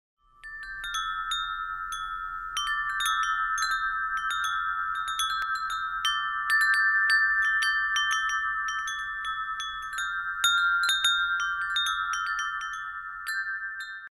Woodstock Chimes - Sounds of the Month, March 2013
Tuned to an ancient Celtic scale, our Woodstock Celtic Chime is the perfect chime for March. Happy St. Patrick's Day!